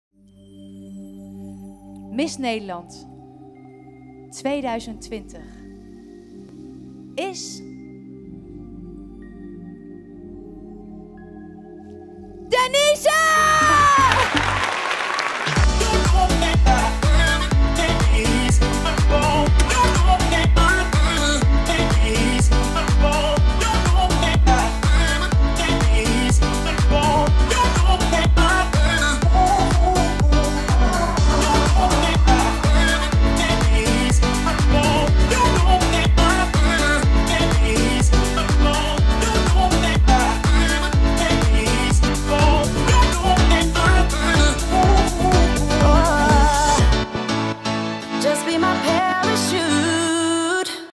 This is Denise - Miss Nederland 2020
Denise Speelman is gekroond tot Miss Nederland 2020, samen met 9 andere finalisten begon ze op 14 juni aan het missen avontuur. Met o.a. de Bootcampweek in Oostenrijk, fotoshoot op een sprookjes achtige locatie en natuurlijk de onvergetelijke finale.

📽 BeemFlights